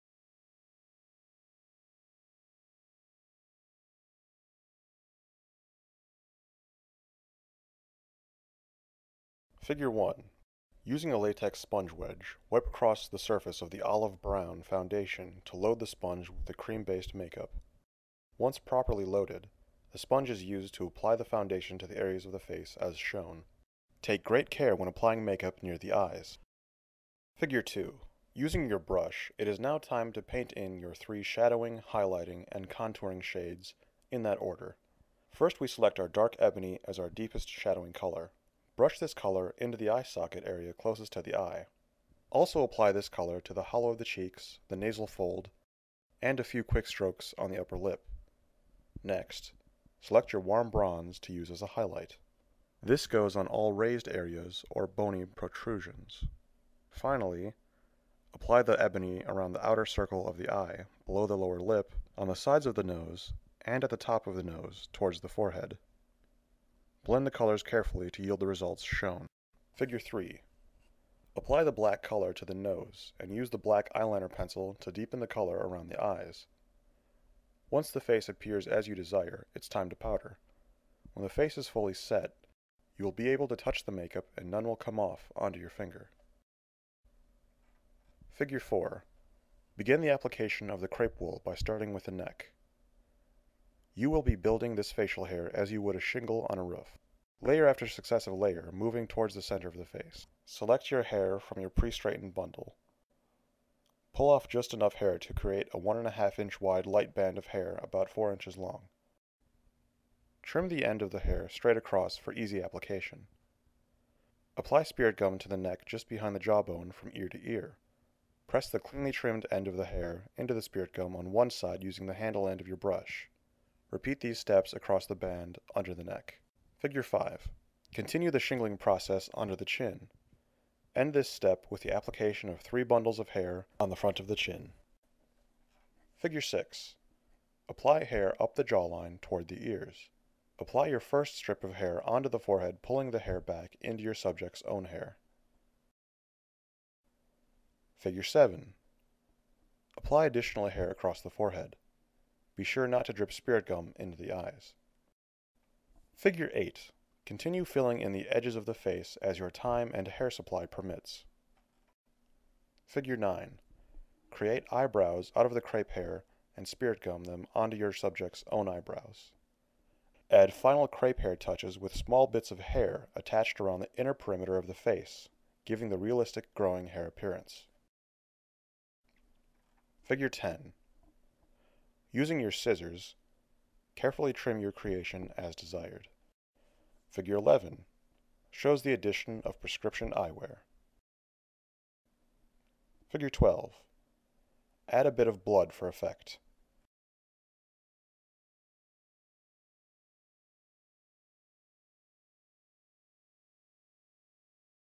Werewolf Kit Slide Show
Graftobian's Werewolf Kit Instructions in slide show format.